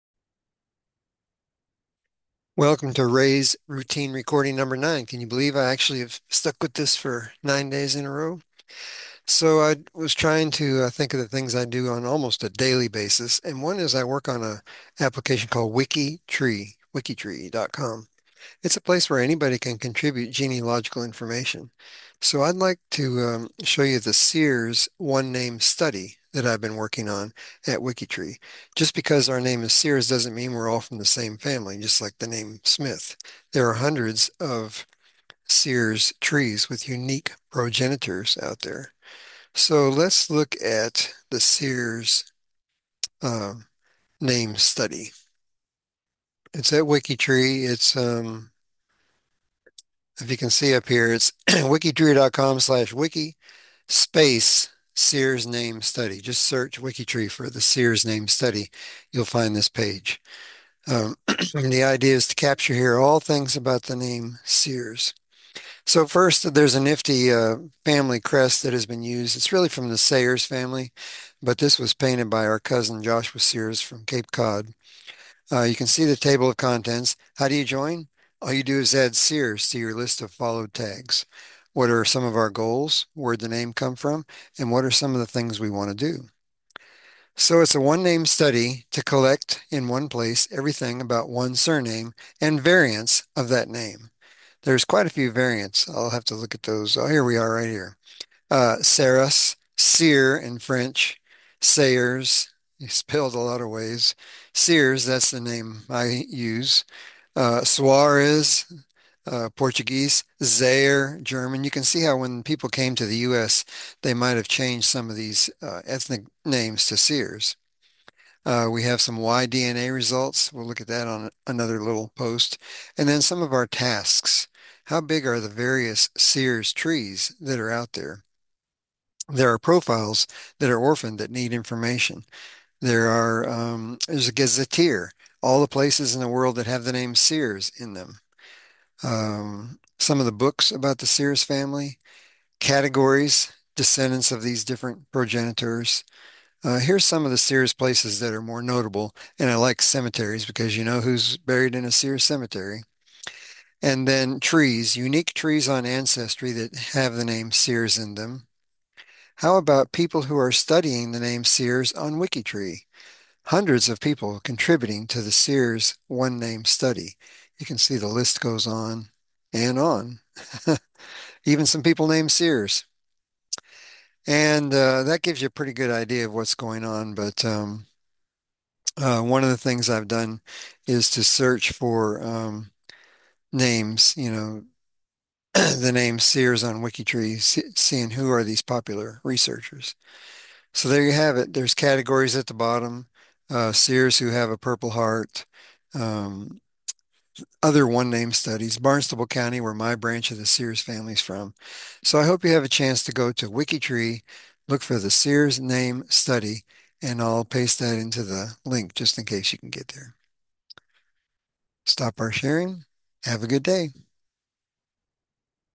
RaysRoutineRecording009- Sears One Name Study
A study of the family name Sears with hundreds of bits of information